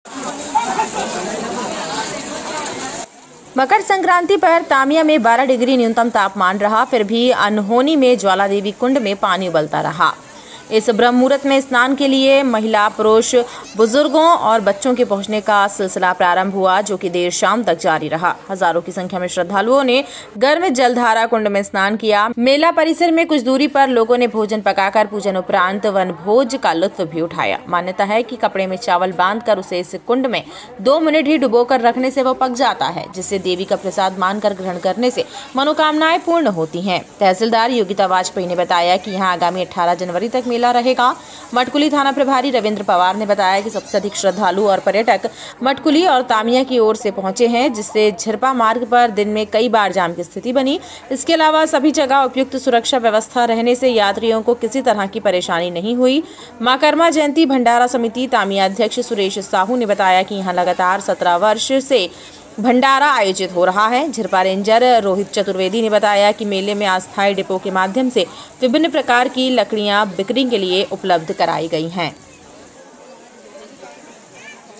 छिंदवाड़ा/झिरपा--अनहोनी गर्म जलधारा कुंड : 12 डिग्री तापमान में भी उबलता रहा पानी
मकर संक्रांति पर हजारों श्रद्धालु कुंड में स्नान करने पहुंचे